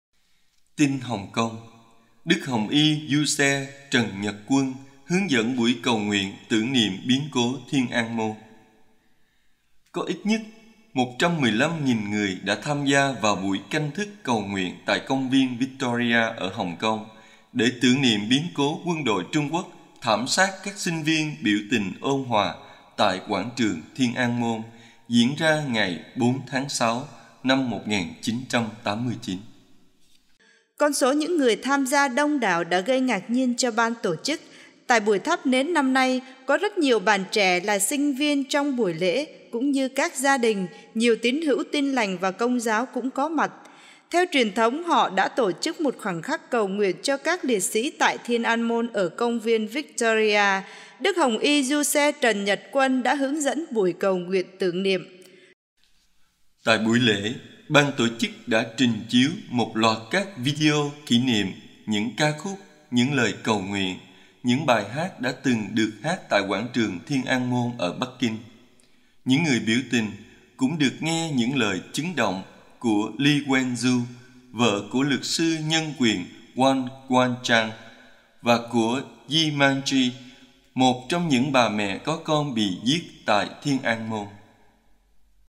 Đức Hồng Y Giuse Trần Nhật Quân hướng dẫn buổi cầu nguyện tưởng niệm biến cố Thiên An Môn
TRÍCH: ĐIỂM TIN GIÁO HỘI HOÀN VŨ TUẦN QUA: 04/06 - 09/06/2018